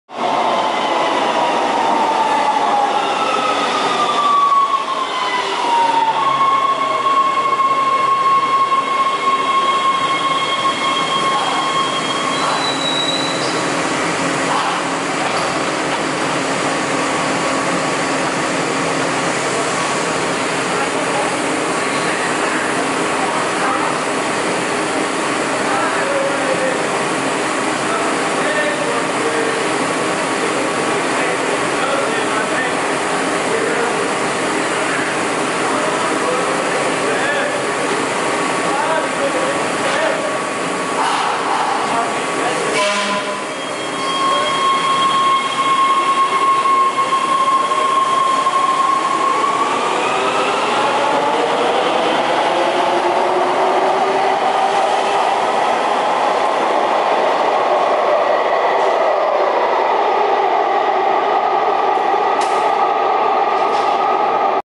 LACMTA Metrorail: Union Station bound A650 B Line Train at Pershing Square
A LACMTA B Line (Red Line) train consisting of Breda A650 EMUs makes a station stop at Pershing Square station. This train is bound for Union Station.
Taken Saturday, August 7, 2021.